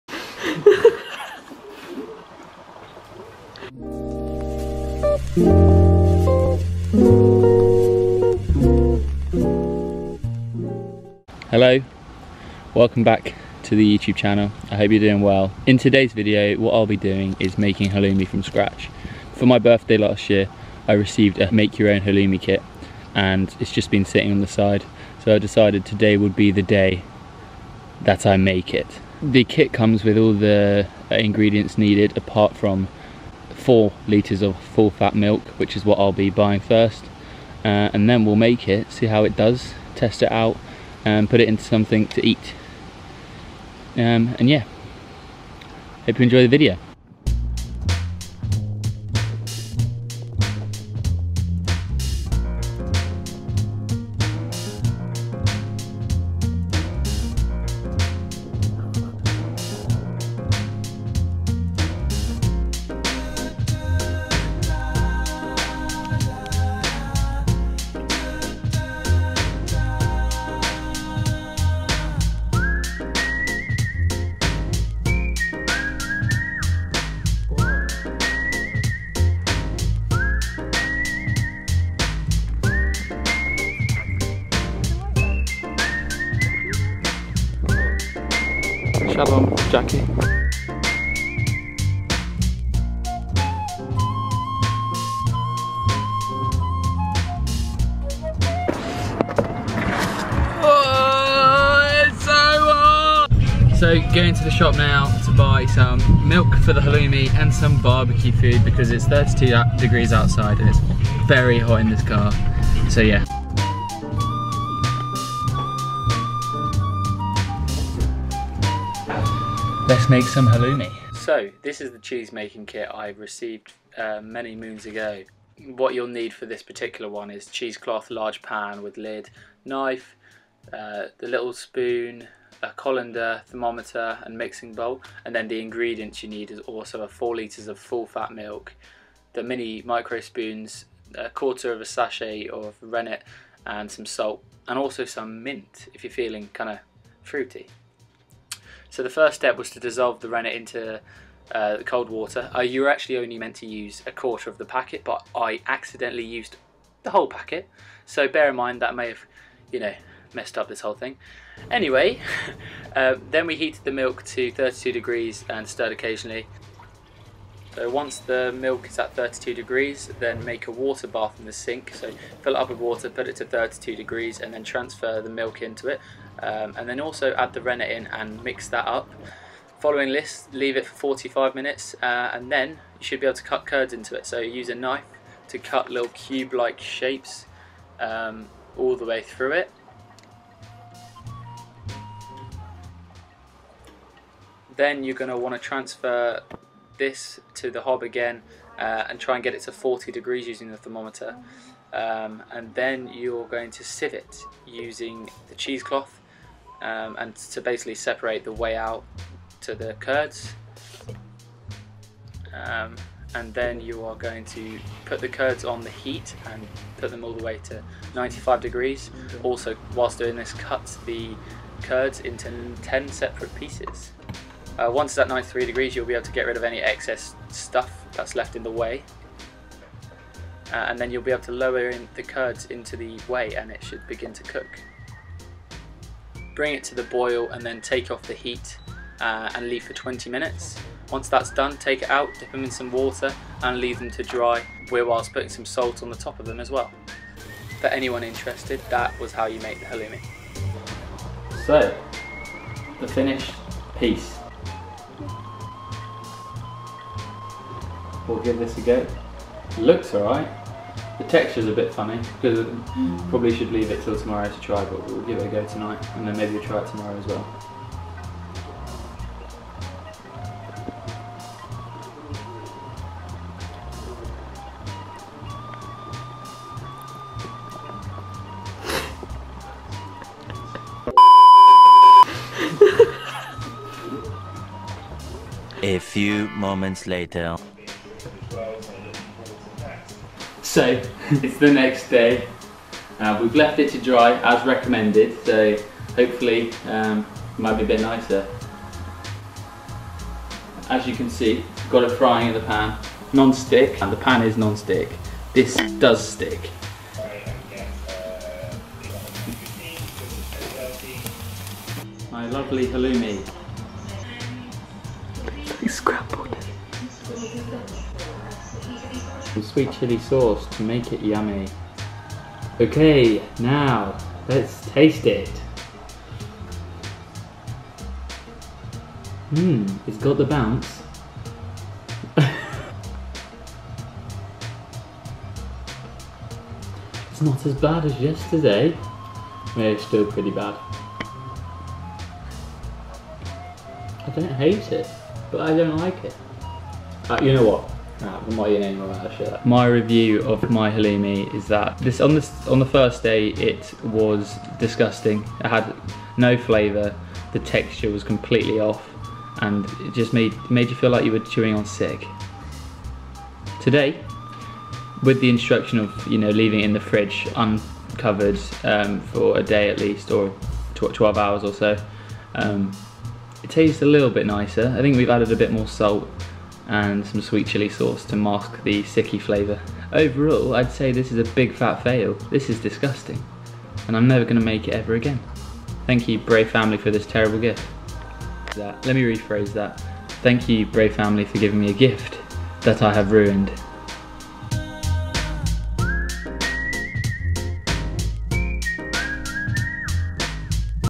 How NOT To Make Halloumi from Scratch
Making Halloumi For The First Time!
I make halloumi! This proved harder than expected but a lot of fun!